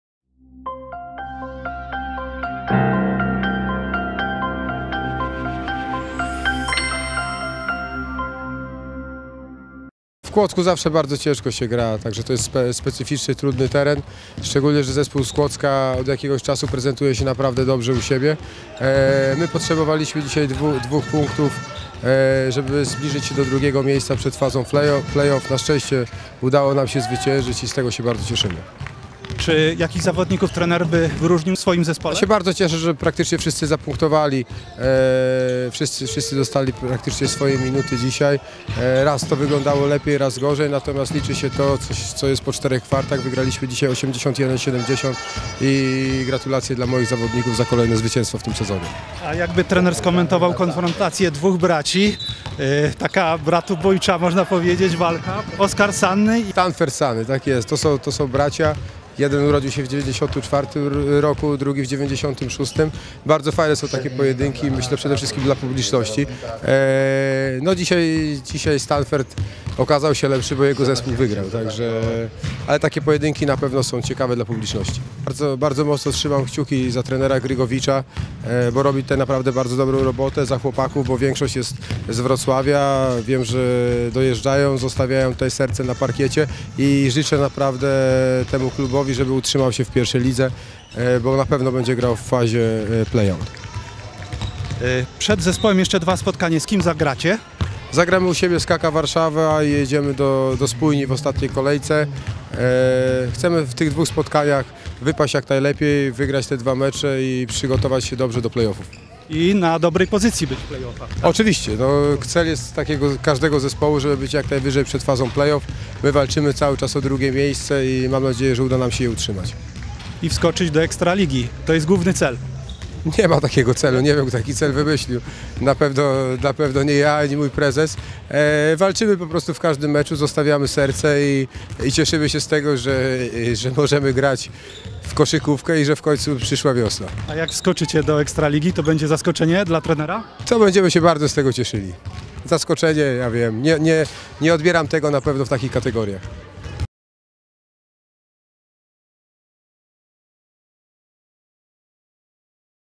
Wywiad pomeczowy z trenerem Łukaszem Grudniewskim
Wywiad z  trenerem Jamalex Polonia 1912 Leszno Łukaszem Grudniewskim  po spotkaniu pomiędzy Zetkamą Doralem Nysą Kłodzko a Jamalex Polonia 1912 Leszno.